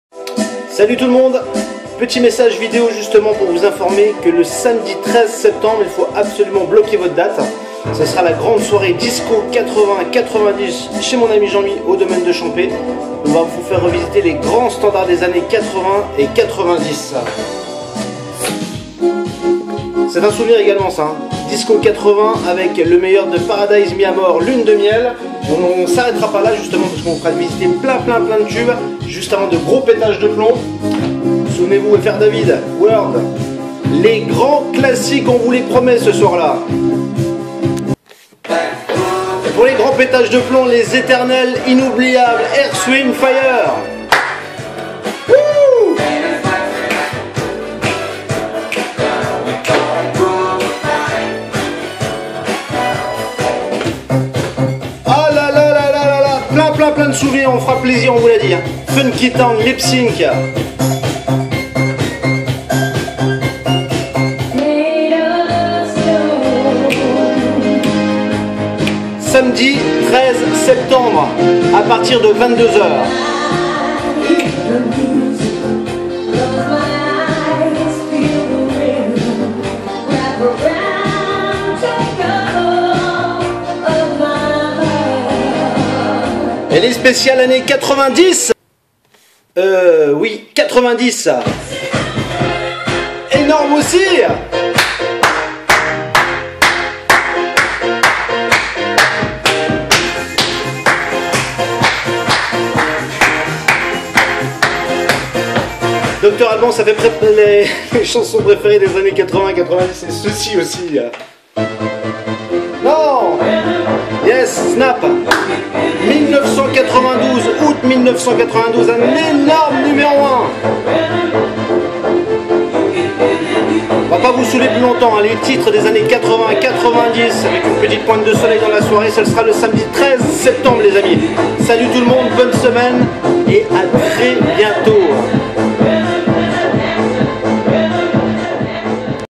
Soirée Disco 80 90 au Domaine de Champé à Bussang Le Samedi 13 Septembre 2014
Dans une folle ambiance revivez le meilleur des années 80 et 90 avec les perles du disco afin de rajeunir le temps d'une soirée et revivre une époque inimitable... Le rendez """JUSTE""" incontournable de la rentrée les amis... SAMEDI 13 SEPTEMBRE AU DOMAINE DE CHAMPE A BUSSANG A PARTIR DE 22H !!!